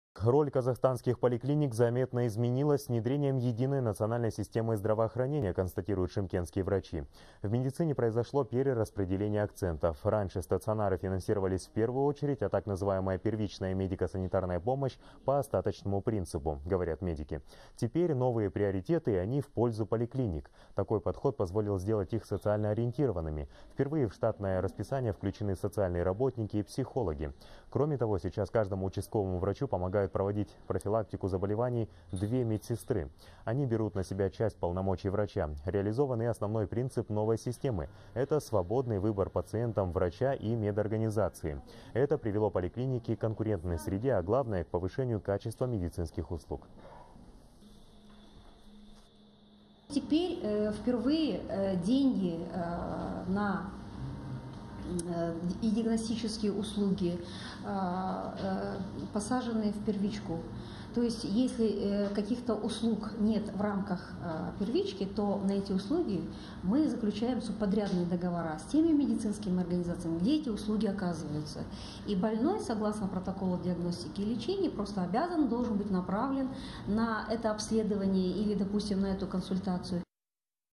Единая национальная система здравоохранения улучшает роль поликлиник РК
Роль казахстанских поликлиник заметно изменилась с внедрением единой национальной системы здравоохранения.